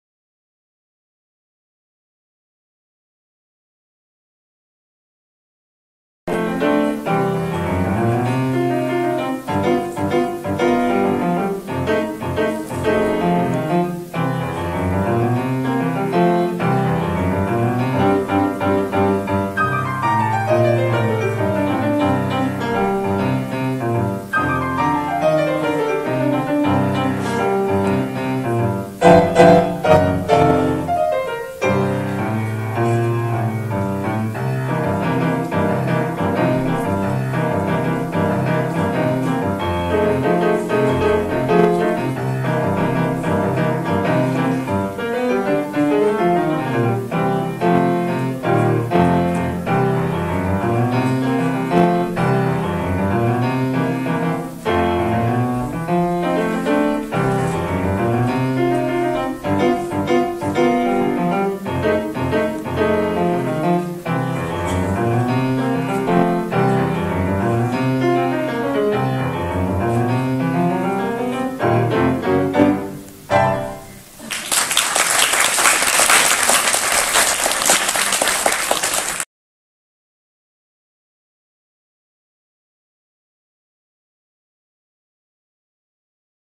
С Новым Годом! Или дубль - 3
С  Новым  Годом,  друзья!

Вас  поздравляет  юный  пианист!